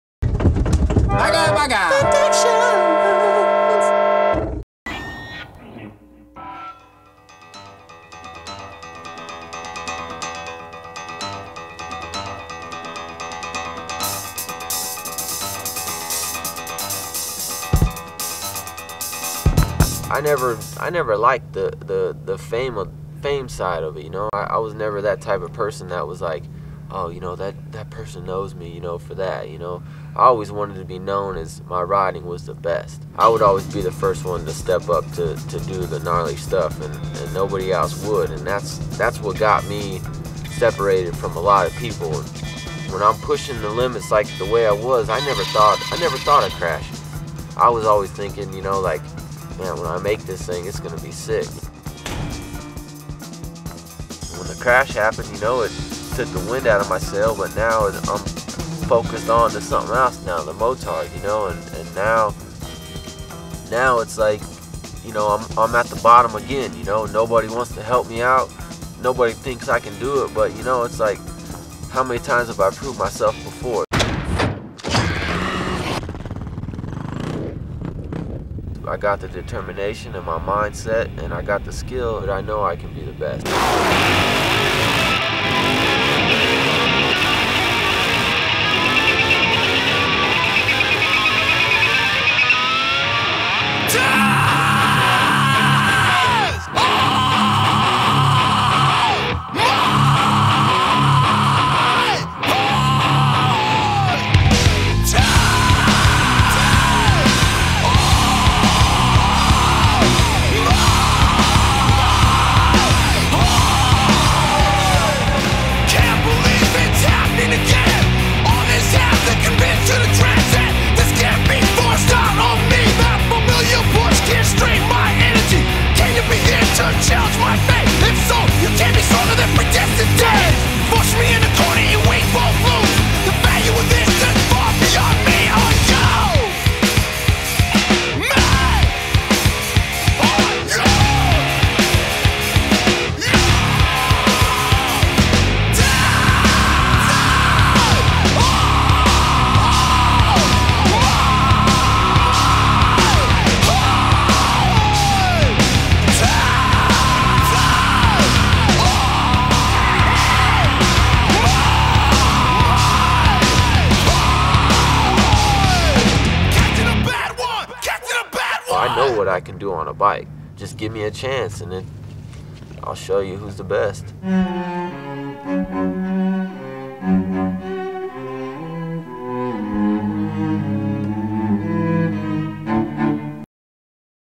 SUPER SKETCHY SUPERMOTO w/ Mike Cinqmars (2003)
I loved filming with Cinq. After he broke his back, he got into Super-Moto and so I filmed this segment to help him announce it. Some of those highway shots were straight up sketchy as we had no permits or permission.